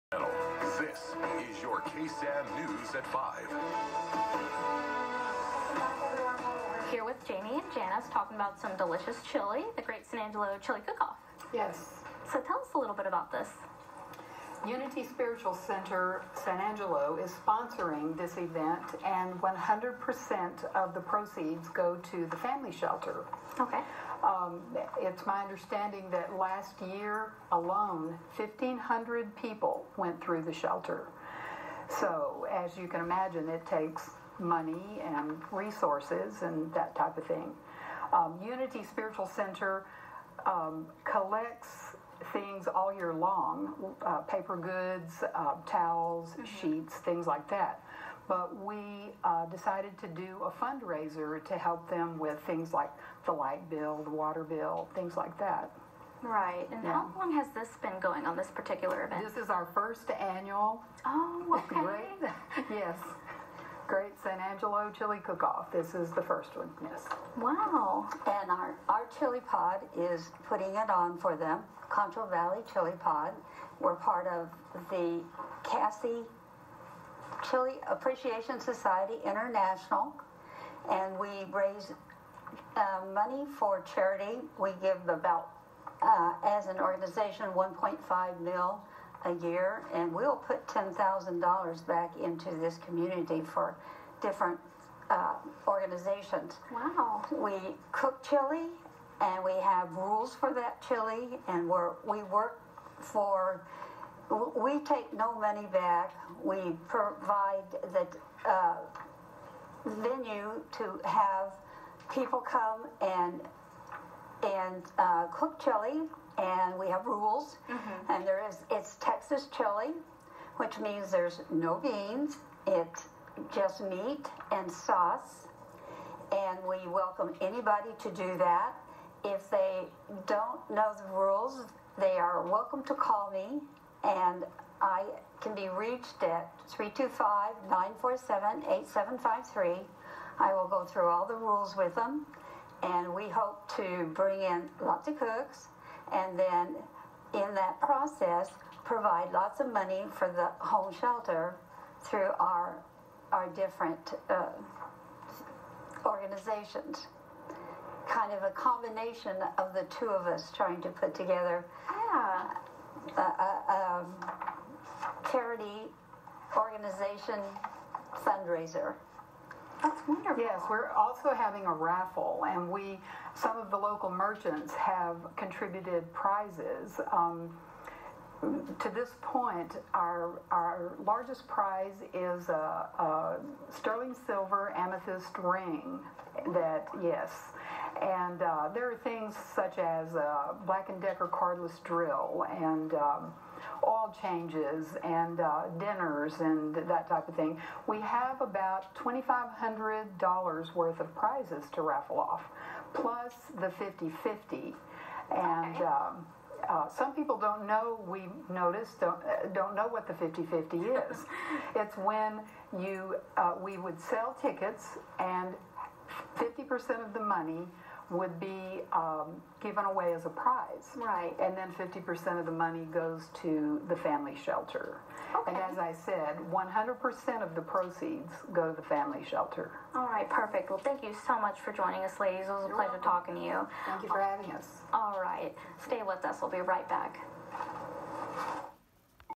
The Great San Angelo Chili Promotion!
This is the second in a series of promotions on television regarding the great San Angelo chili cook off. It features Reverend Janie Kelley from unity spiritual center in San Angelo and Janice Miller from the San Angelo Chil pod which is one of the groups from the chili appreciation society international.